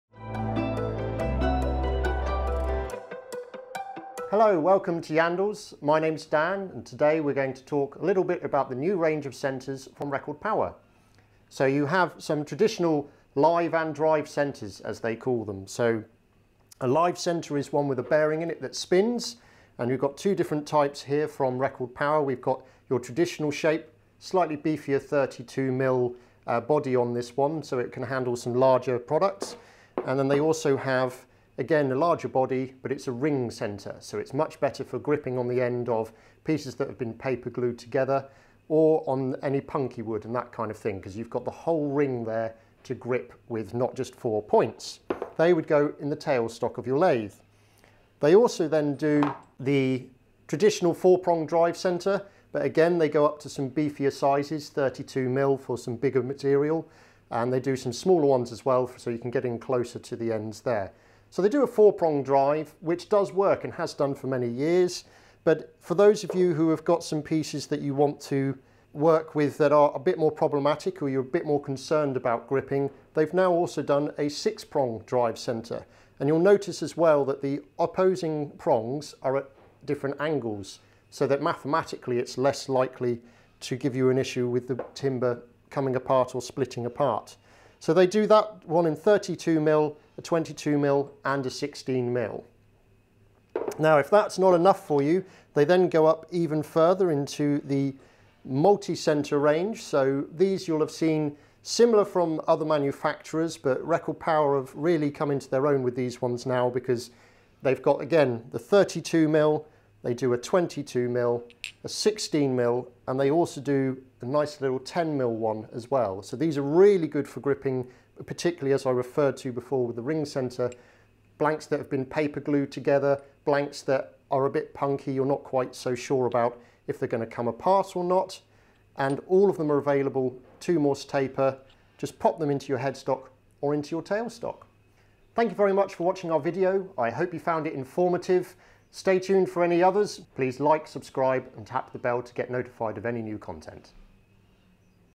The NEW Hawk And Falcon Wood Lathe Centres from Record Power
A brief look at the new Hawk And Falcon Wood Lathe Centres from Record Power.

4 Prong drive centre
The traditional 4-prong drive centre is designed to give a strong hold to timber using the principle of opposing tooth profiles. The central point of the tool secures the drive centre in place, the four teeth around it grip the timber securely and the opposing tooth pattern ensures the wood fibres are pulled together to reduce the chance of splitting.

The 22 mm diameter drive centre is ideal for use on large square or round profiled spindle stock and the smaller 16 mm drive centre for small to medium-sized spindle stock.

6 Prong Drive centre
The design of the 6-prong drive centres offers an enhanced grip to timber using the principle of opposing tooth profiles. The central point of the tool secures the drive centre in place, the six teeth around it grip the timber securely and the opposing tooth pattern ensures the wood fibres are pulled together to reduce the chance of splitting. Having two more teeth than the traditional 4-prong drive centres, they are ideal for use on hardwoods and larger pieces of wood.

The 32 mm diameter drive centre is ideal for holding side-grain bowl blanks. It is also suitable for holding natural-edged bowls between centres, particularly if seated inside a 32 mm diameter recess, for extra support from the recess walls.

The 16 and 22 mm drive centres are ideal for holding spindle work.

Multi tooth Drive Centres
The multi-tooth drive centres feature a ring of teeth around the circumference of the tip, with a central drive point to give accurate positioning. This design gives an extremely strong grip, thanks to the many points of contact in the timber, which also minimises the risk of splitting the timber.

As the grip is spread across many small teeth rather than 4 or 6 prongs, it is possible to grip the timber sufficiently just by using the clamping pressure of the lathe tailstock - no need to drive the tips into the wood with a mallet.

Another benefit of this design is that your work can be removed from the lathe and accurately repositioned, using the centre point and ting of teeth as an accurate guide.

These drives also give a fantastic grip to paper-joint wood blanks, as the ring of teeth grip on many points across the drive’s circumference across both sections of wood on either side of the joint.

Falcon Live Centre

This live centre features two high quality sealed-for-life bearings, giving incredibly smooth rotation and the 60º point is optimised to give the best grip possible from a single point of contact. It is ideal for use on spindle work and can also be used to support headstock mounted bowls and larger vessels from the tailstock.

Falcon Ring Centre

This ring centre features two high quality sealed-for-life bearings, giving extremely smooth rotation. The ring design gives a larger area of contact than a traditional live centre for an increased grip. This makes it ideal for supporting small or brittle timbers and works very well with paper-jointed blanks, giving a strong grip to both pieces of timber either side of the joint.

Another great advantage of this design is that workpieces can be removed and refitted to the lathe accurately, thanks to the positive location of the centre point and ring, which can be used for easy registration.